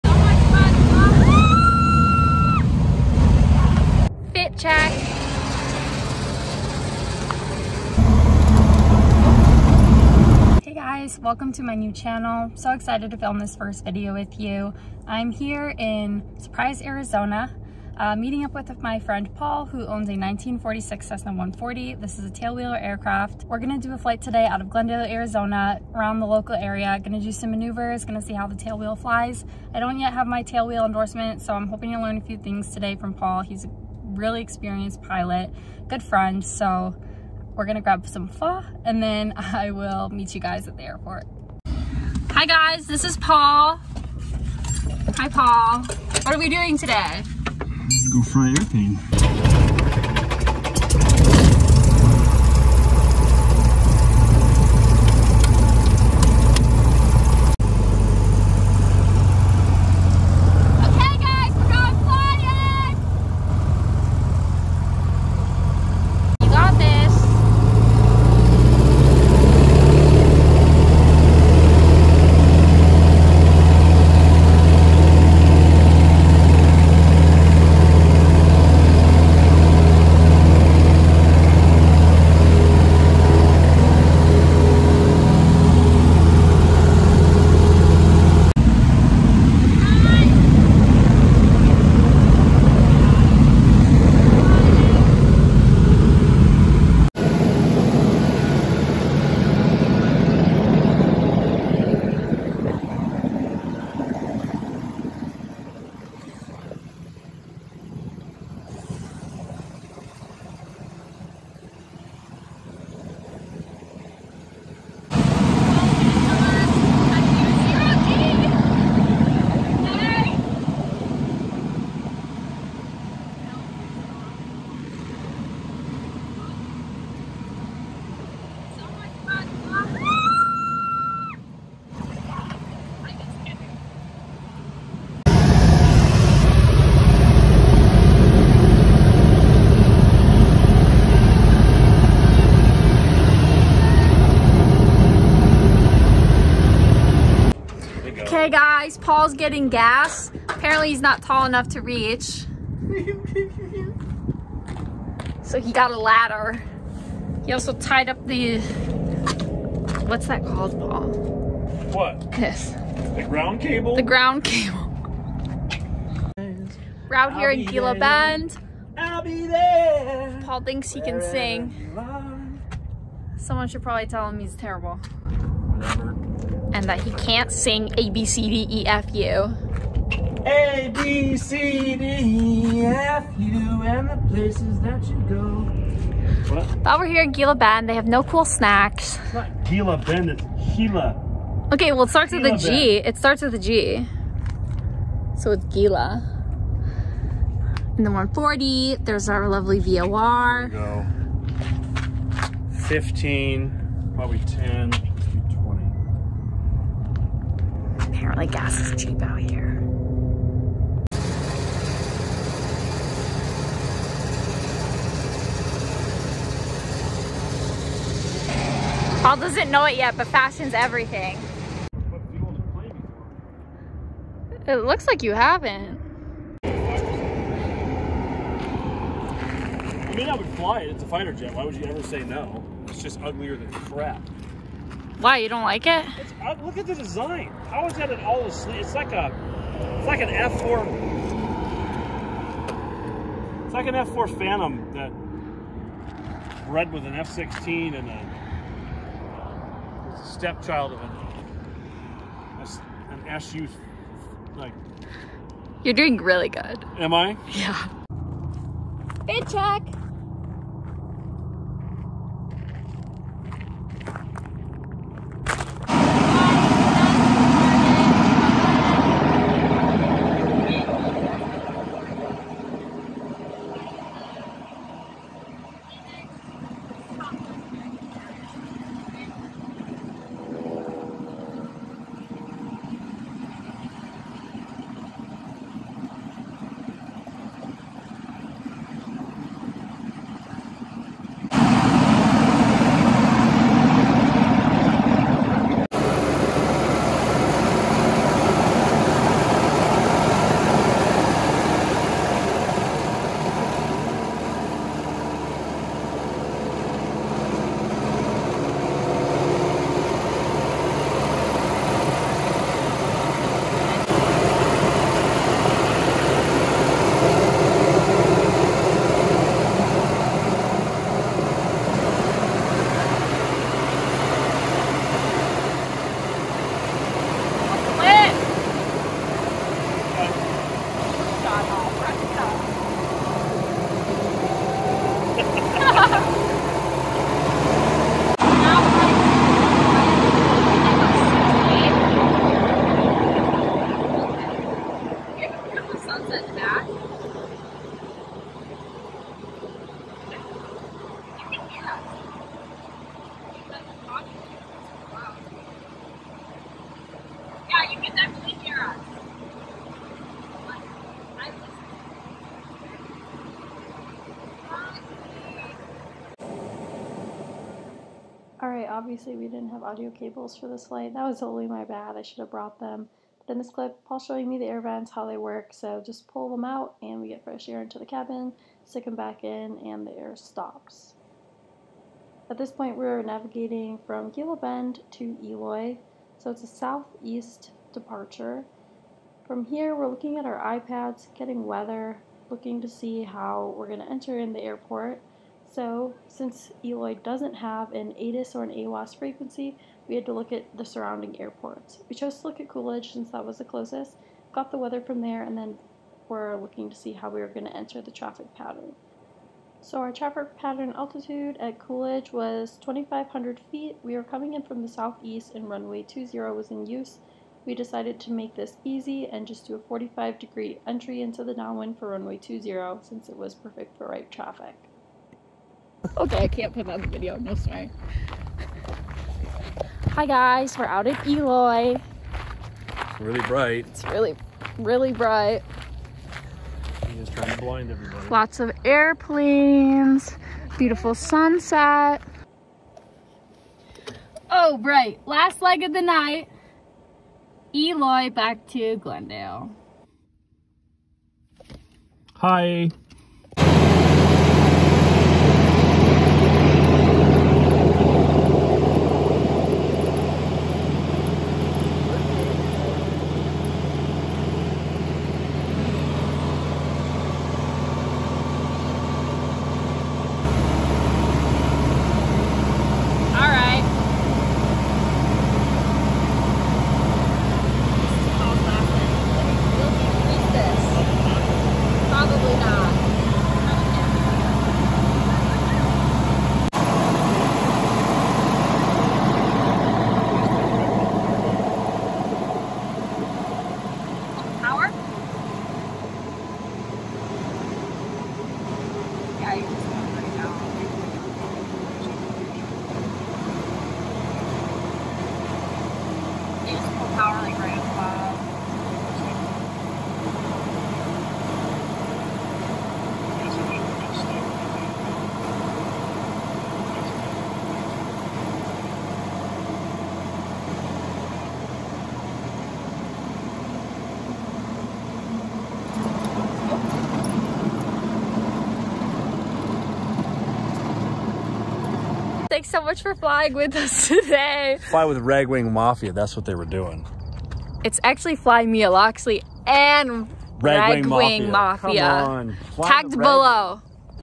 Cessna 140 with Ragwing Mafia
Route: KGEU - E60 - E63 - KGEU

So excited to post my first video featuring this flight with Paul, aka Ragwing Mafia! This was the second time we got to go up in his 1946 Cessna-140! Paul is currently a 206, 207, Caravan, and a Citation CJ 2 in Arizona and a great CFI and pilot.

We went over some maneuvers like steep turns, and he showed me a few things about how tailwheel's fly and two new airports I was unfamiliar with. We also got cleared for the east transition through KPHX class bravo airspace on our way back which was so awesome since we got to fly right over the KPHX runway's at 4,500' MSL. This was also awesome opportunity to improve my familiarity with traditional flight instruments rather than glass.

I had so much fun on this flight and am looking forward to learning more so I can earn that tailwheel endorsement.

**P.S. sorry we forgot audio cables**